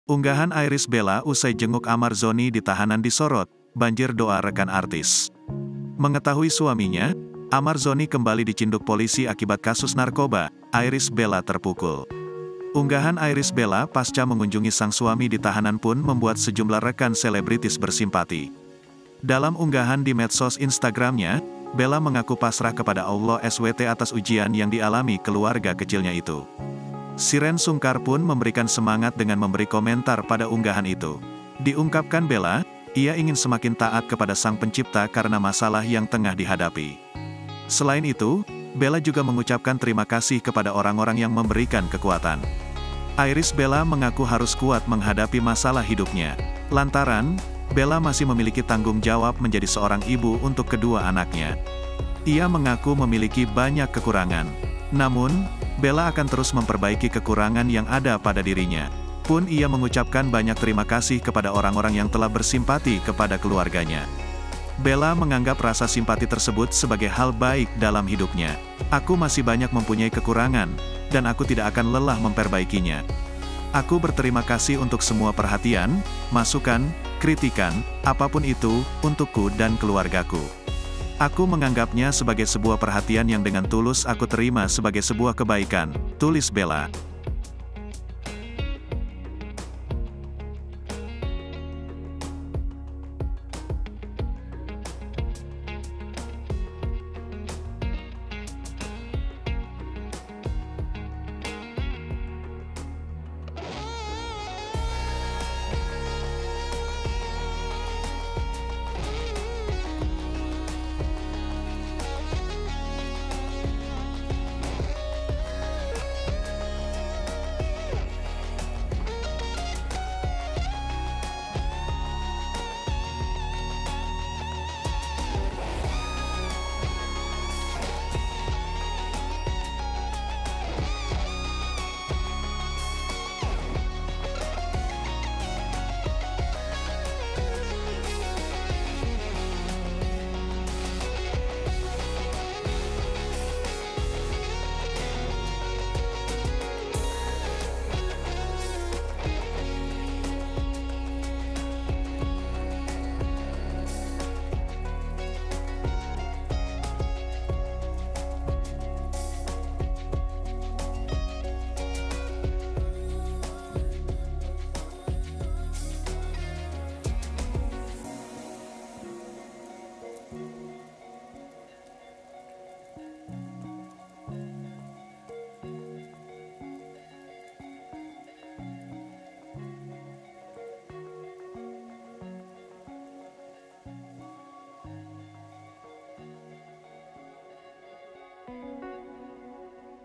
UNGGAHAN IRISH BELLA USAI JENGUK AMMAR ZONI DI TAHANAN DISOROT, BANJIR DOA REKAN ARTIS
TRIBUNLAMPUNG.CO.ID - Unggahan Irish Bella usai jenguk Ammar Zoni di tahanan disorot, banjir doa rekan artis.